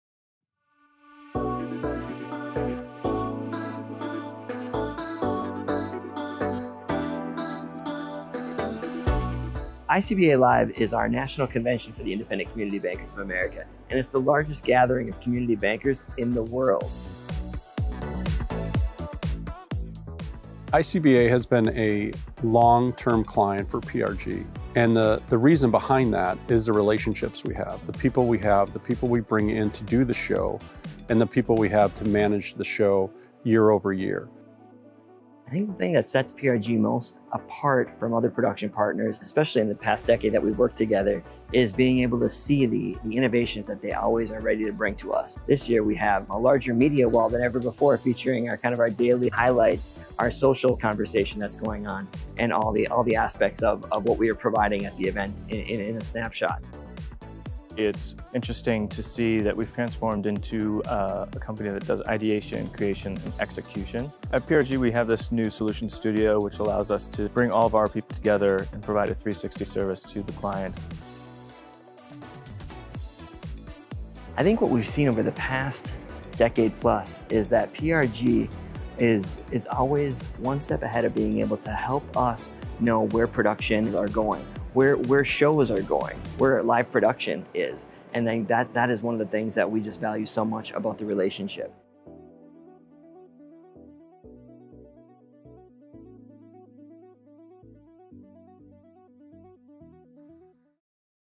ICBA Live 2022
PRG’s long-term client, the Independent Community Bankers of America (ICBA), gathered in San Antonio, Texas this year for its annual convention. The largest event in its community, ICBA partnered with PRG to deliver 360° solutions for their general session and breakout rooms for the 4-day event. Join Jim Mastey, ICBA's Senior VP Conventions and Meetings, Tom Berberet, PRG Account Executive and Nick Shafer, PRG Project Manager as they take you through the making of.